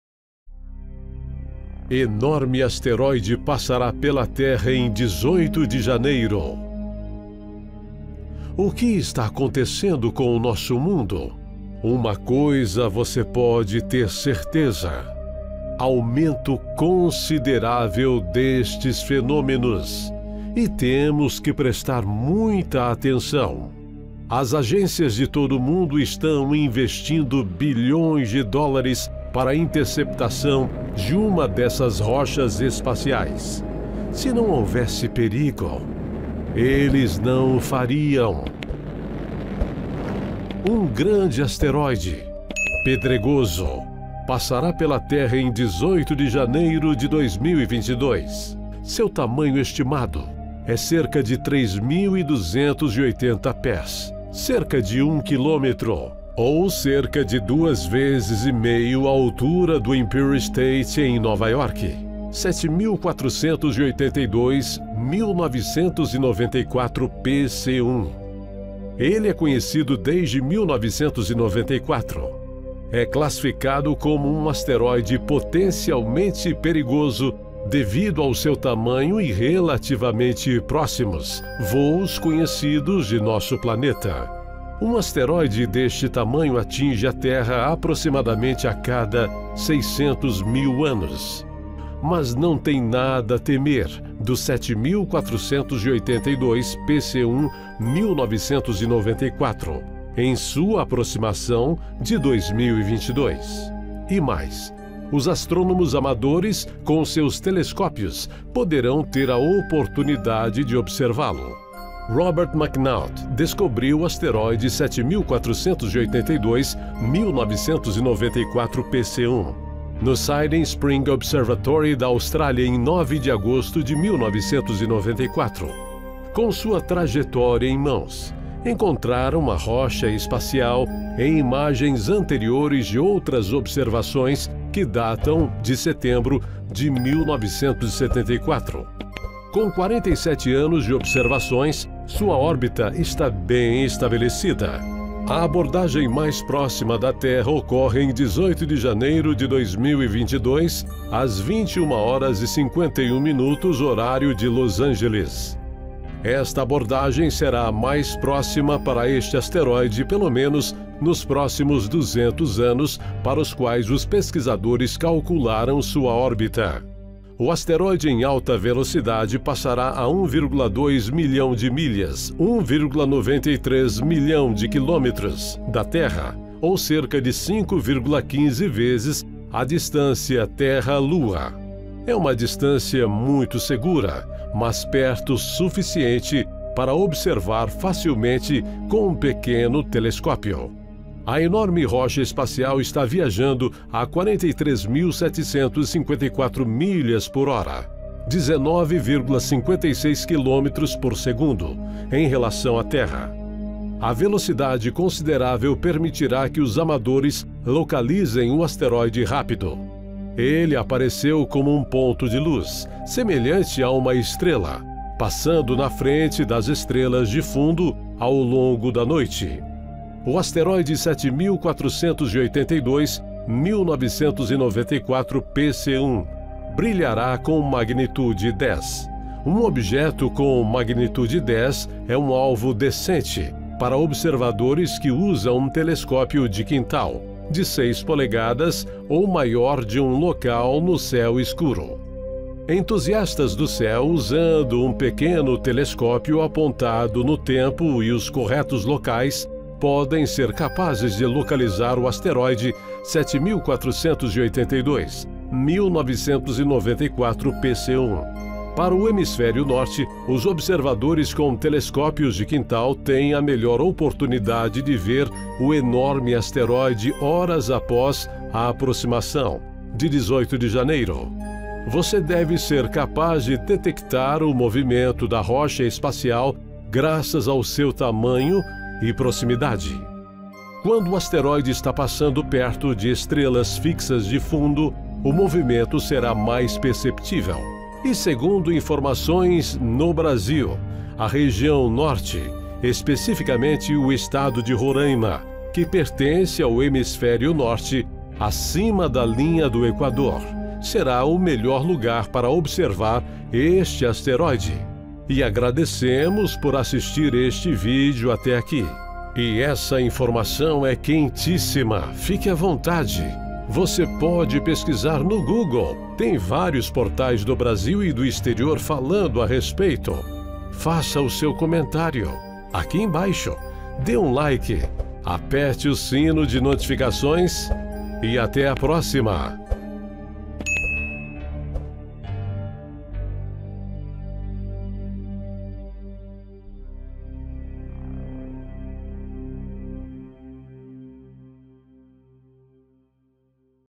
ASTEROIDE DE 1 kM DE DIAMETRO PASSARA PERTO DA TERRA
🅷🅸🆂🆃ó🆁🅸🅰/🅲🆄🆁🅸🅾🆂🅸🅳🅰🅳🅴🆂/🆄🅽🅸🆅🅴🆁🆂🅾
O que está acontecendo com o nosso mundo ?
Uma coisa você pode ter certeza do aumento consideravel destes fenômenos, e temos que presta muita atenção...
As agências de todo mundo estão investindo bilhões de dólares para interceptação de uma destas rochas espacias, se não ouve-se perigo eles não o fariam.

Um grande asteroide pedregoso passará pela Terra em 18 de janeiro de 2022. Seu tamanho estimado é de cerca de 3.280 pés (cerca de 1 km), cerca de 2 1/2 vezes a altura do Empire State Building  em Nova York - (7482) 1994 PC1 - ele é conhecido desde 1994. É classificado como um Asteroide Potencialmente Perigoso devido ao seu tamanho e relativamente próximos voos conhecidos de nosso planeta.
Produção Voz em Ação
Narração Dalton Jr

Ｎａｒｒａçãｏ Ｄａｌｔｏｎ Ｊｒ
ｈｔｔｐｓ：／／ｗｗｗ．ｄａｌｔｏｎｌｏｃｕｔｏｒ．ｃｏｍ．ｂｒ